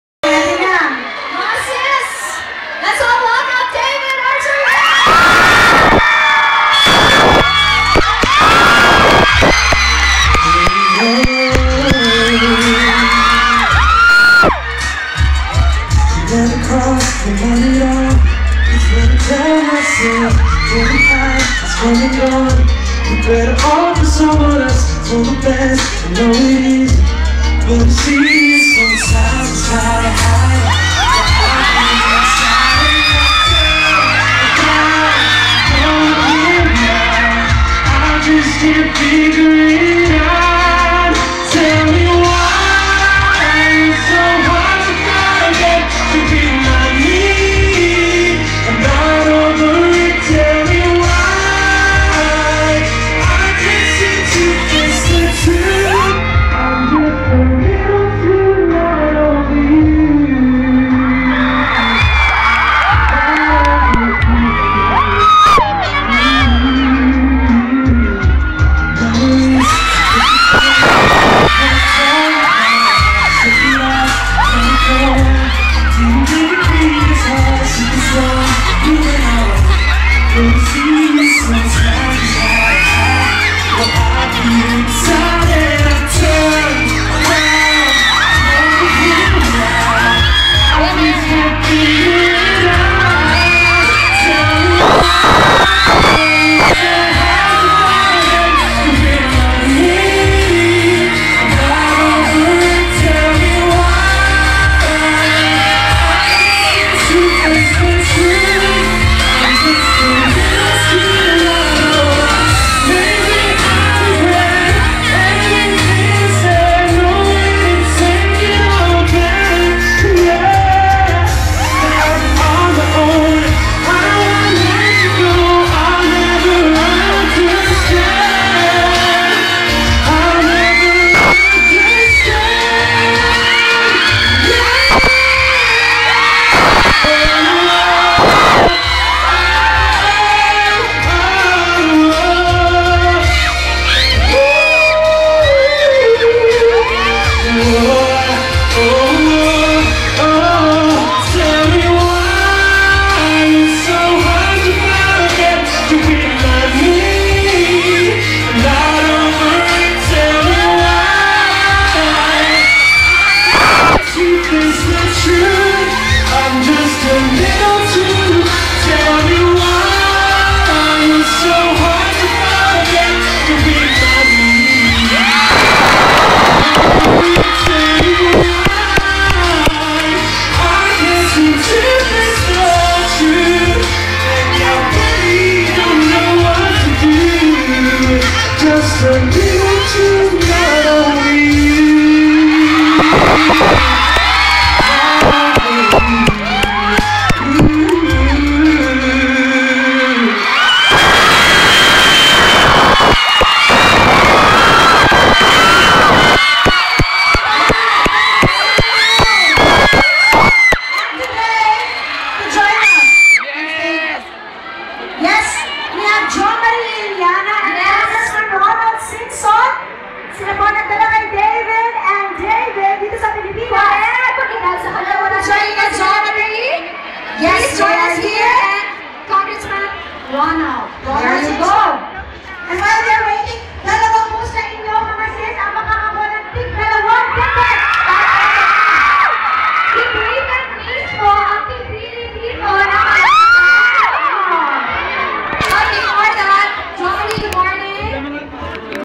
David Archuleta performs in the philippines
me, mariefel and jelly went in the morning show in the philippines called SIS because we knew that david archuleta will perform on the stage that day. i decided to upload it just to know everyone that david performed energetic. this is one of his performances in the philippines. im sorry if the video was a little bit blurry. Filipinos like me love him so much!!... I LOVE YOU DAVID!!!